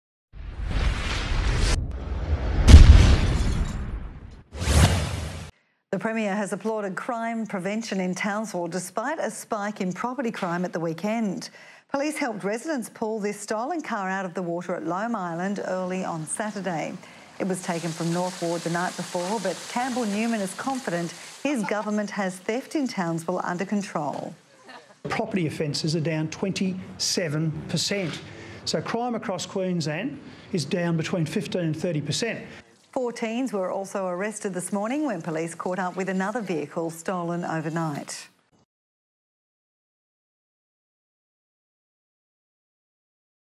Tcad 7 News - Premier Applauds Crime Prevention In Townsville
The premier has applauded crime prevention in Townsville despite a spike in property crime over the weekend